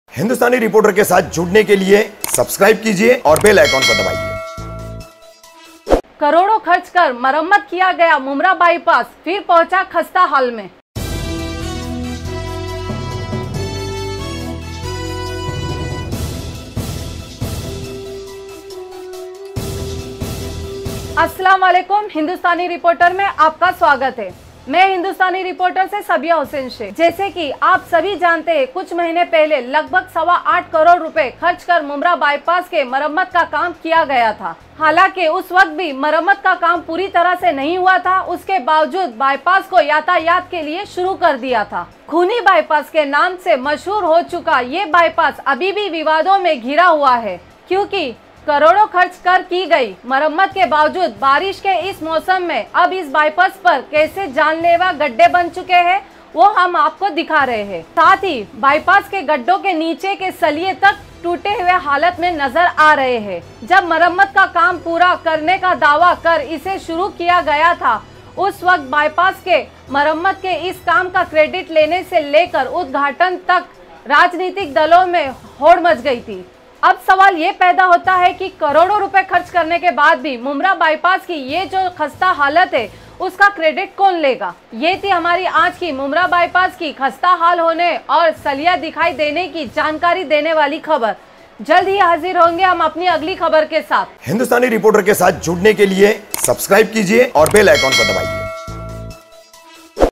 Croro Rupe Kharch Kar Marammat Kiya Gaya Mumbra Bypass Phir Pahuncha Khaste Haal Me
Bypass Pe Ban Chuke Hai Janlewa Gadde,  Dikhai De Rahe Hai Lohe Ke Tute Hue Saliye